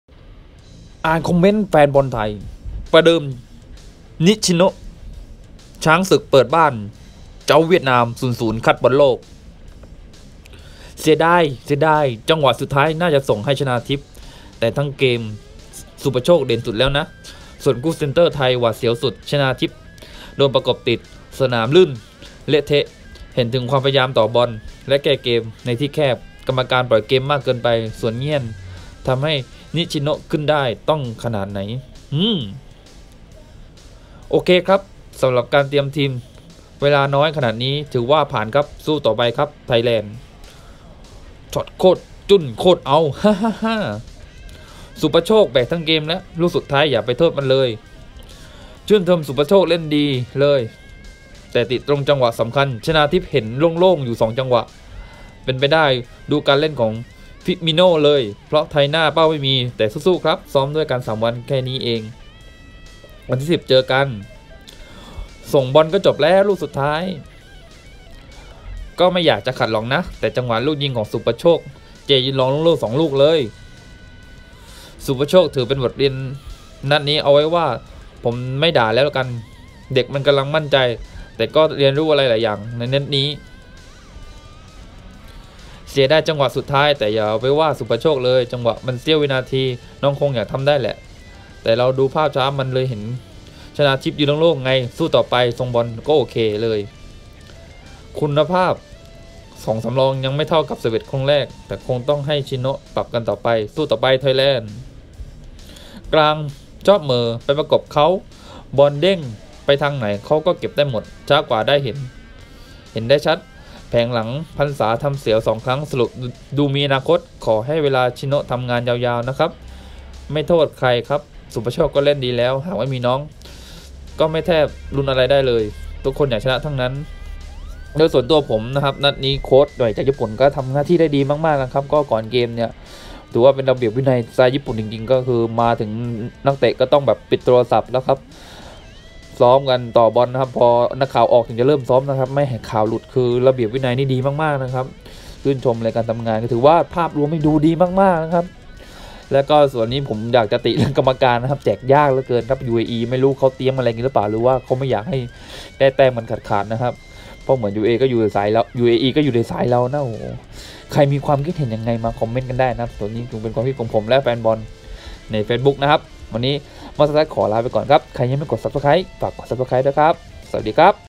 อ่านคอมเม้นแฟนบอล เกือบดีแล้ว ! บอลไทย ทำได้แค่เปิดบ้านเจ๊า เวียดนาม 0-0, ศุภโชค ฟอร์มแจ่ม
นิชิโนะประเดิมคุม! ทีมชาติไทย เจาะไม่เข้าเจ๊า เวียดนาม 0-0 คัดบอลโลก